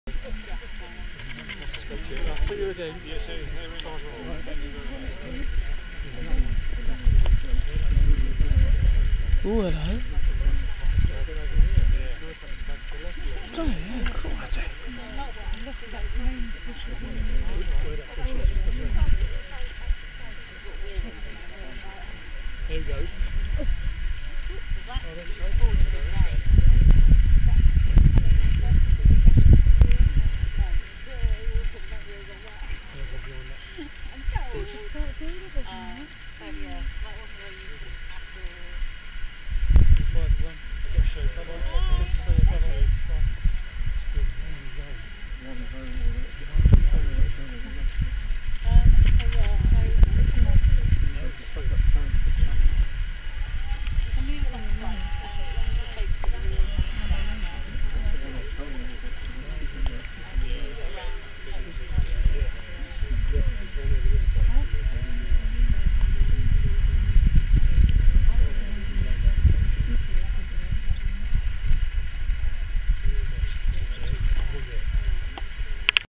Red Deer Stag and Hinds, Westleton Heath, Minsmere, Suffolk
A red deer stag checks the females (hinds) in his harem if they are in season, ready to mate.

Video taken on Saturday 1 October 2011.